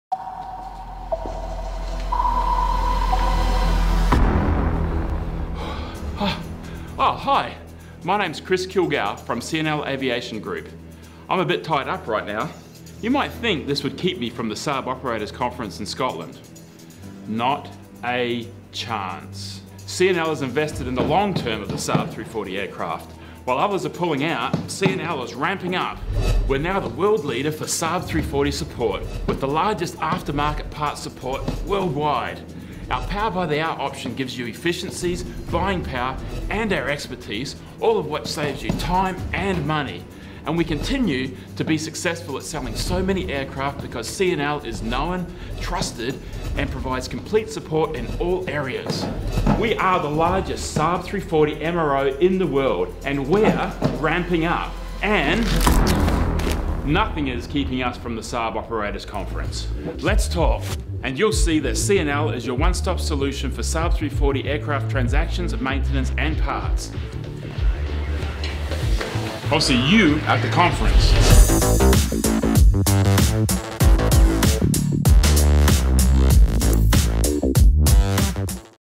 C&L Aviation Group | Got To Get To Glasgow (Part I)
Find out if our CEO Chris Kilgour can escape and make it to this year’s Saab Global Operators Conference.
for more information about our part support and services.